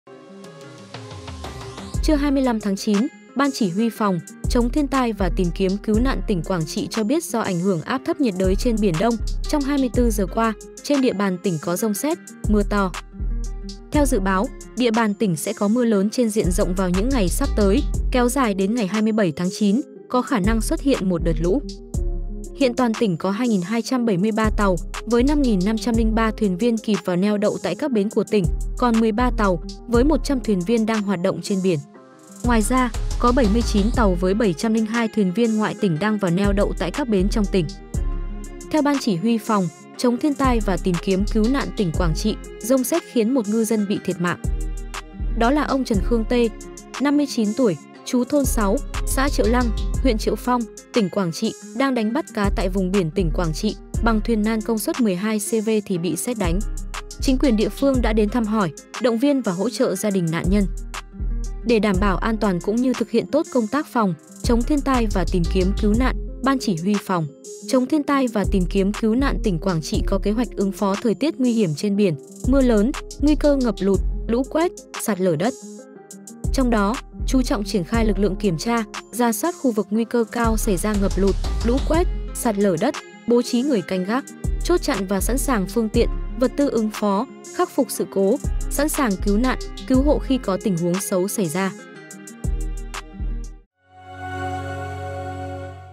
Một ngư dân bị sét đánh tử vong khi đang đánh cá trên biển
Trưa 25-9, Ban Chỉ huy Phòng, chống thiên tai và tìm kiếm cứu nạn tỉnh Quảng Trị cho biết do ảnh hưởng áp thấp nhiệt đới trên biển Đông, trong 24 giờ qua, trên địa bàn tỉnh có dông sét, mưa to.